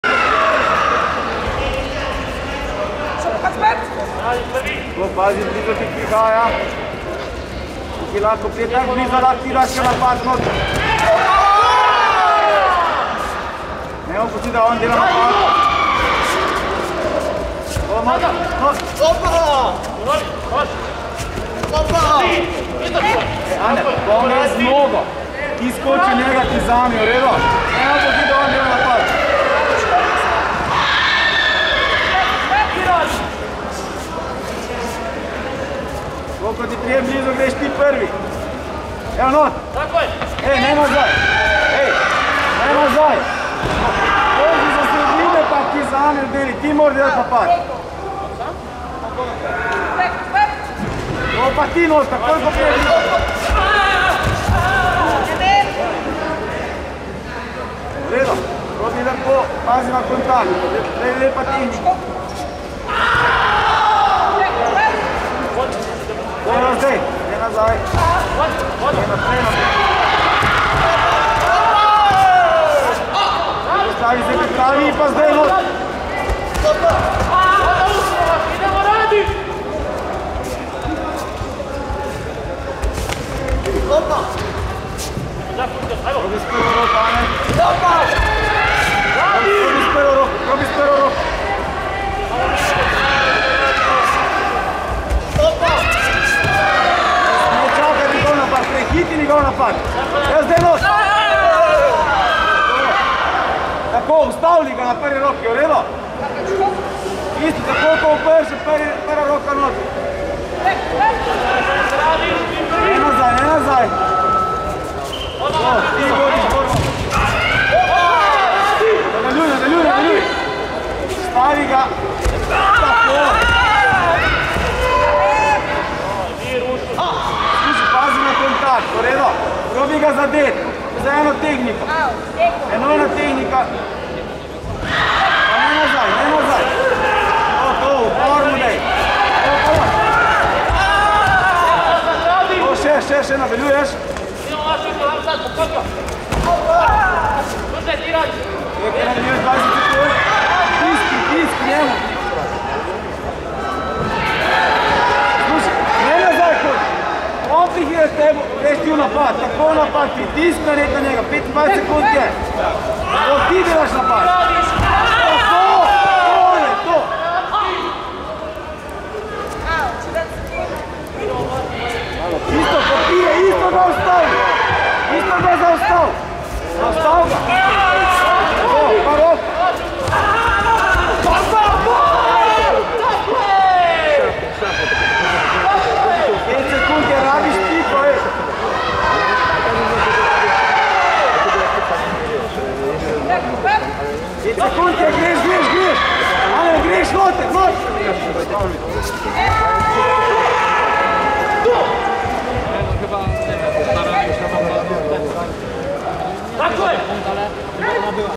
Karate1 Youth League - Porec 2021 - Junior Kumite Male -68 kg
Junior Kumite Male -68 kg - Repechage
AMIDŽIĆ ANES (BIH) - ČRNČEC ANEL (SLO)   2 : 1